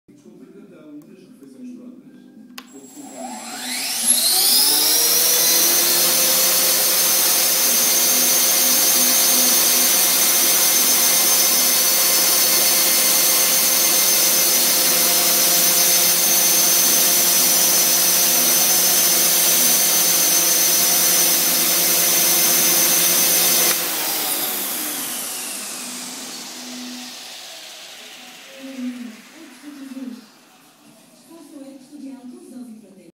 Trex 450 with GY520
My first run-up with the heli fully assembled.
It's a Trex 450 with a HK450 canopy.

The GY520 proved to be an excellent gyro. Very stable. I just need to correct the little tail wagging.

Now it's a little bit more training on the simulator and give the bird it's first flight.